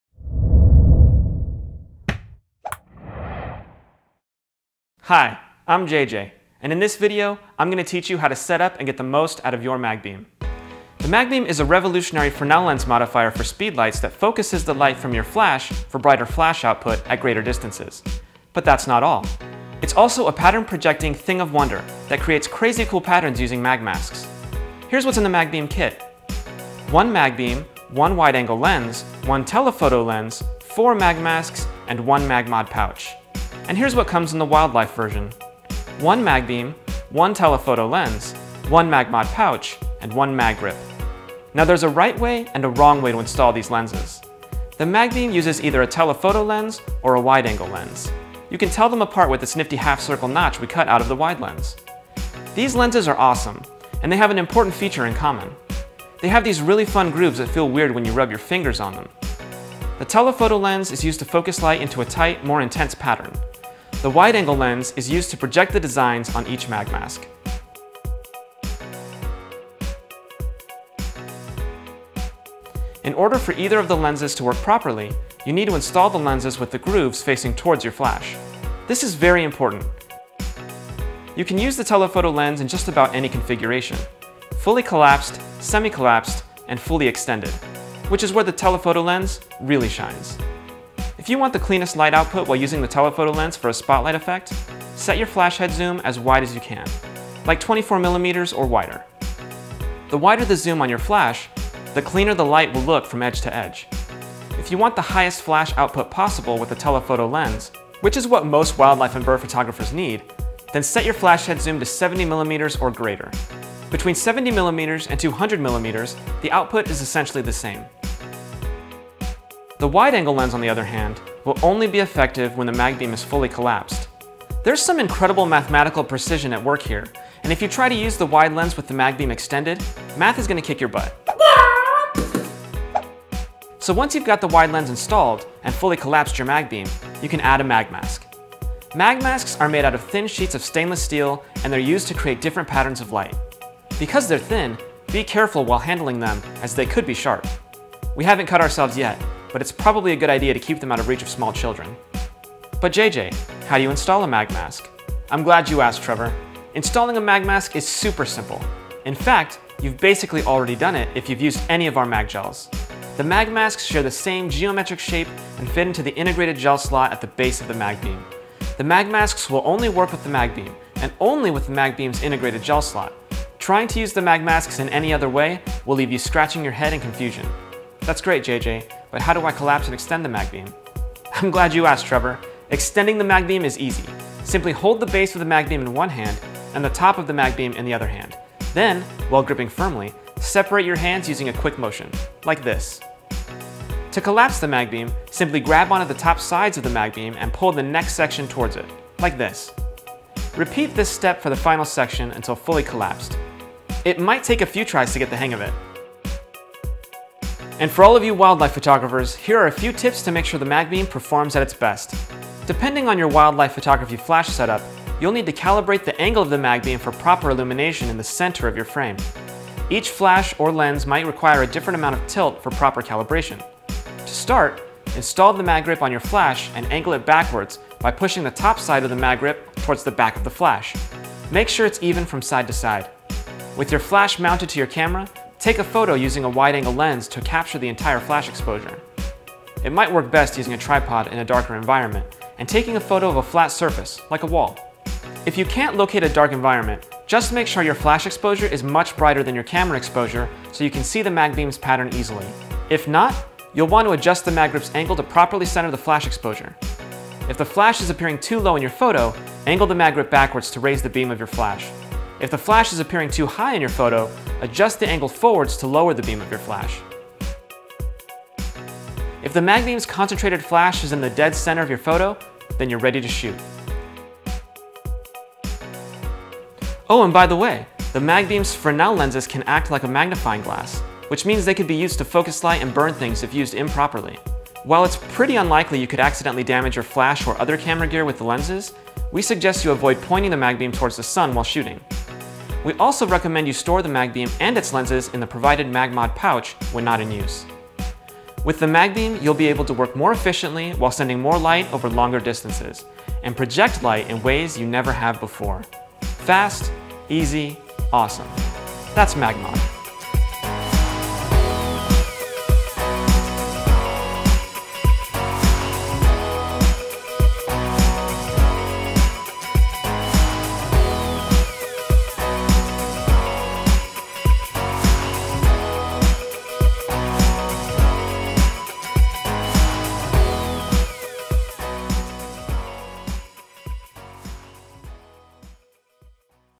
How to use the MagBeam
JJ from MagMod is here to help explain the details of using and getting the most out of your MagBeam!
Click "Show More" to... See more... :)

FIND US ON THE INTERNETS